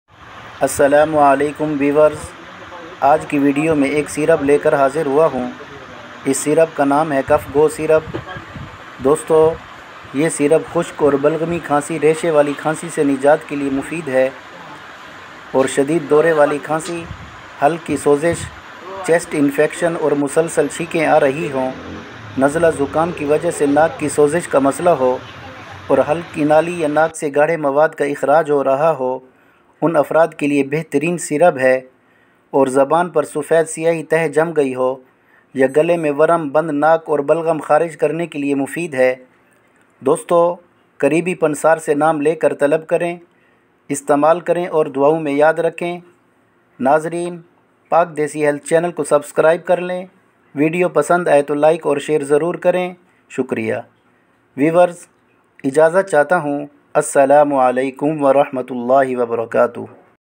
khansi ka ilaj cof go syrup خشک اور بلغمی کھانسی کا علاج
Friends, in today's video, I have brought a syrup. Its name is Cuff Go Syrup. Friends, this syrup is useful for relieving dry and phlegmatic cough. Fibrous cough. If you have a problem with nasal congestion, a sore throat or discharge of thick mucus from the nose, it is best for people who are having a runny nose and a white or black layer on the tongue. Thanks

دوستو آج کی وہڈیو میں ایک سیرپ لیکر حاضر ھوا ھوں اس کا نام ہے کف گو سیرپ دوستو یہ سیرپ خشک اور بلغمی کھانسی ریشے والی کھانسی سے نجات   کیلے مفید ہے شدید  دورے والی کھانسی حلق کی سوزش چیسٹ انفیکشن اور مسلسل چھیکیں  آرھی ھو  نزلہ زکام کی وجہ سے ناک کی  سوزش کا مسلہ ھو حلق کی نالی یا ناک سے گاڑھے مواد کا اخراج، ھو رھا ھو
 ان افراد کیلے بیترین  ہے اور  زبان پر سفید یا سیاہی  تہہ جم گاٸ ھو گلے میں ورم،بند ناک ، اور بلغم خارج کرنے کیلے  مفید ہے شکریہ